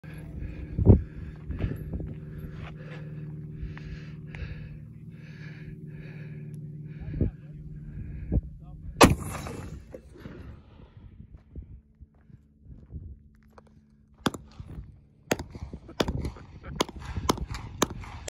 1” black powder cannon firing
Black powder Cannon
If 400 grains is good, 600 is mo betta